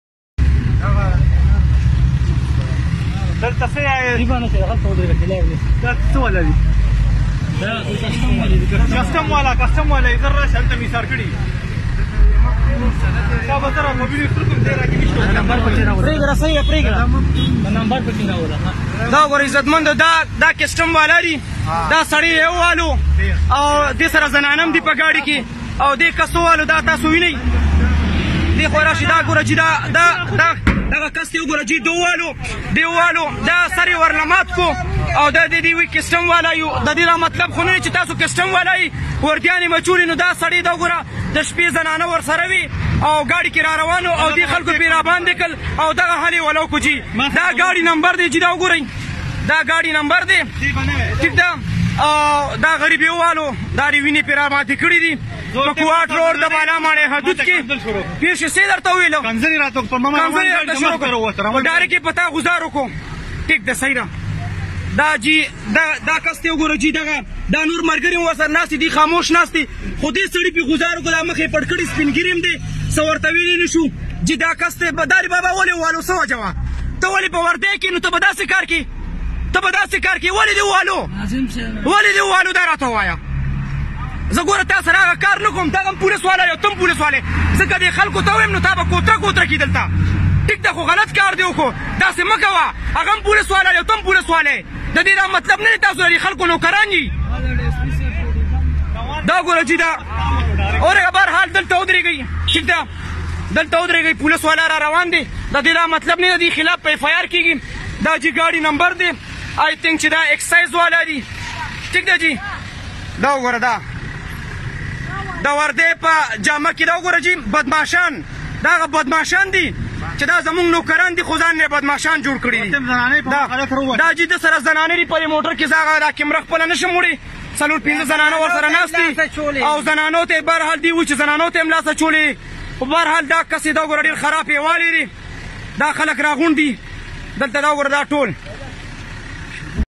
Police fight video
پشاور :تھانہ بہانہ ماڑ ی کے حدود کوہاٹ روڈ پر کسٹم اہلکاروں کی بدمعاشی،اپنی ہی پیٹی بند بھائی پولیس اہلکار جو کہ اپنے الٹو گاڑی میں گھر جا رہے تھے جس کے ساتھ گاڑی میں خواتین بھی موجود  تھیں کو مار مار کر لہولہان کردیا یاد رہے کہ کسٹم اہلکاروں نے الٹو گاڑی میں موجود پولیس اہلکار کو راستہ نہ دینے پر لہو لوہان کر دیا پولیس نے موقع پر پہنچ کر کسٹم اہلکار کو گرفتار کر کے حوالات میں بند کر دیا اور زخمیپولیس اہلکار  کو ہسپتال منتقل کر دیا گیا